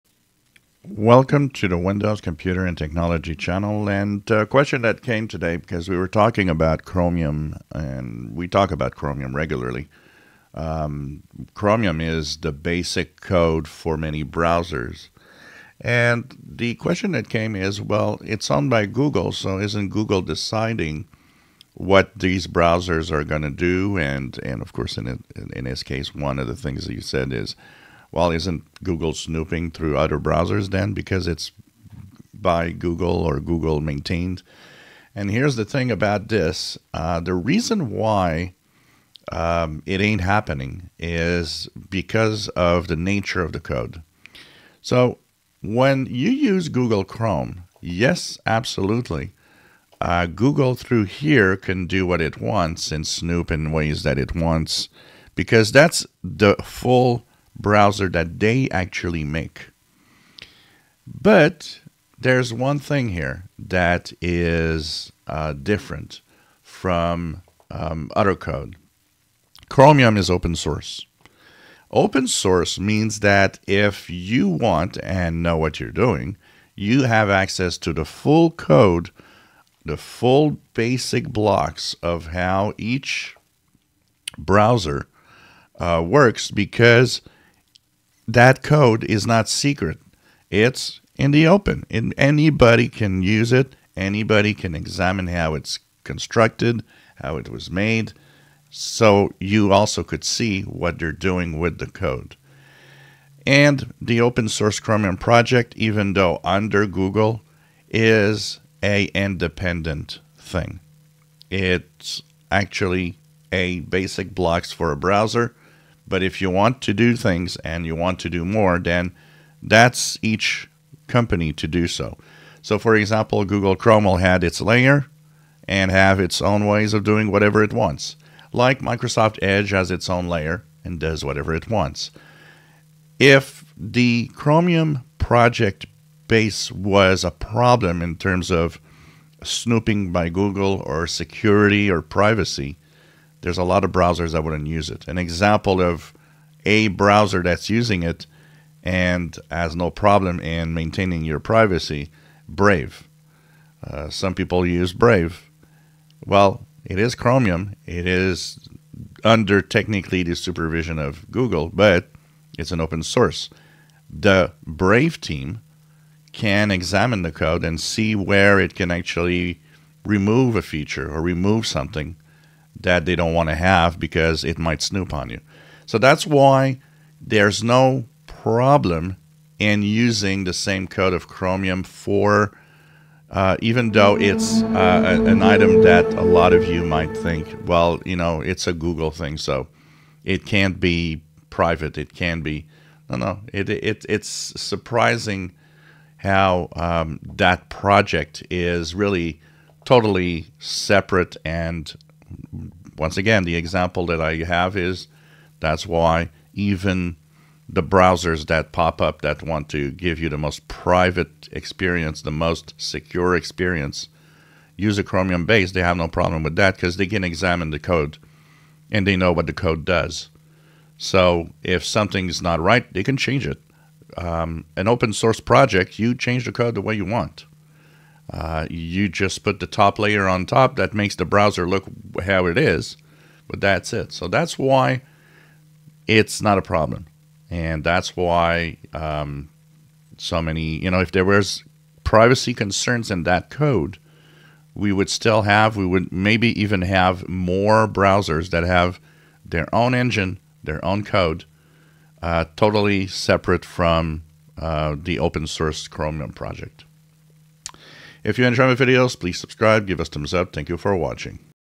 Chromium browsers and privacy concerns with Google owning it
Chromium project is open source, anyone can examine the code and see what it does